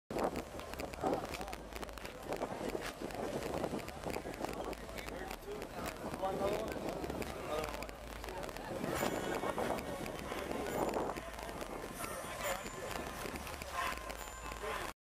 Dodge Challenger on 32" Forgiato Rims!
Dodge Challenger sitting on 32's at the Xtreme Kustoms Car Fest.